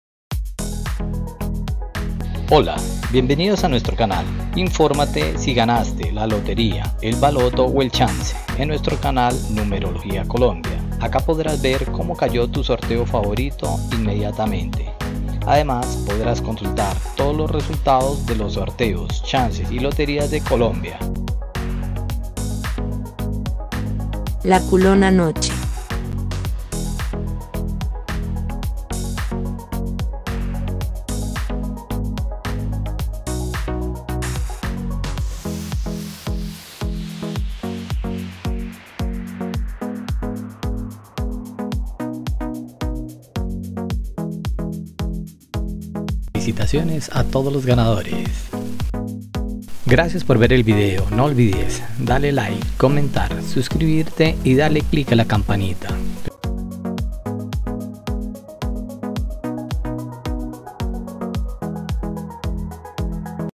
Resultado CULONA NOCHE del jueves 07 de octubre de 2021
Hola buenas noches, te presentamos los resultados de las loterías y chances en Colombia, vamos con el Resultados del Chance Culona noche del jueves 07 de octubre de 2021.

resultados loterias
resultado loteria